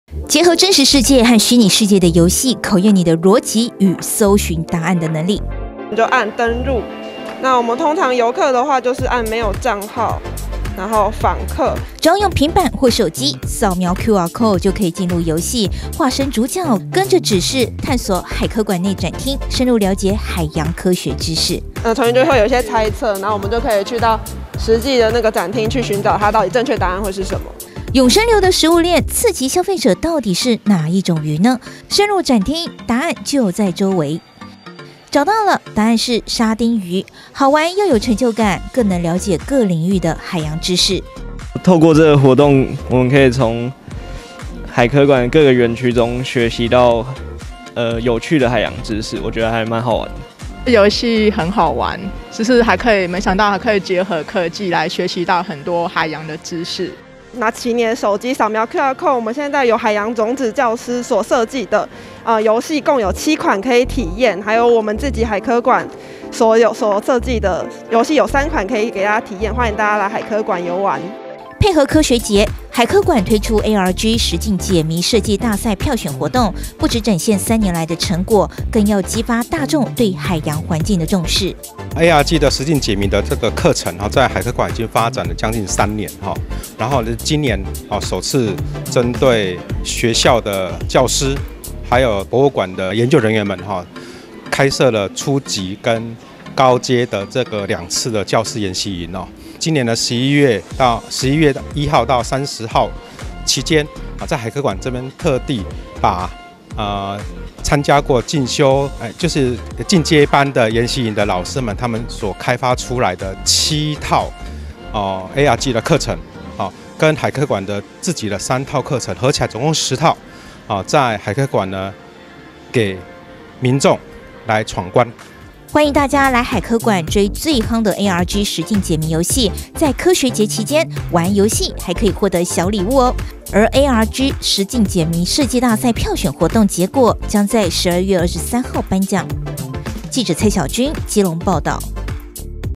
ARG 實境解謎|海科館ARG實境解謎 10款遊戲任你玩 快點追起來
海科館10款ARG遊戲 邀您化身探險家 揭開海洋秘密

(中嘉吉隆新聞 記者蔡曉君/基隆報導) 國立海洋科技博物館推出「ARG實境解謎」活動，結合各展廳內容，推出10款遊戲，讓民眾在遊玩的同時，也能深入了解海洋科學知識。

結合真實世界和虛擬世界的遊戲，考驗你的邏輯與搜尋答案的能力。

只要用平板或手機，掃描QRcode就可以進入遊戲，化身主角，跟著每題的指示，探索海科館內的展廳，深入了解海洋科技知識。

湧升流的食物鏈-次級消費者到底是哪一種魚呢？深入展廳，答案就在周圍。
找到了~~答案是沙丁魚。好玩又有成就感，更能了解各領域的海洋知識。

配合科學節，海科館推出「ARG實境解謎設計大賽票選活動」，展示教育組主任宋祚忠分享三年來的成果，激發大眾對海洋環境的重視。

歡迎大家來海科館追最夯的ARG實境解謎遊戲，在科學節期間玩遊戲，還可以獲得小禮物，而ARG實境解謎設計大賽票選活動結果，將在12月23日頒獎。

Alternate Reality Game

臉書追起來
Home 中嘉- 吉隆有線電視

趣聞不漏接
吉隆官網(中嘉新聞)

喜歡看電視?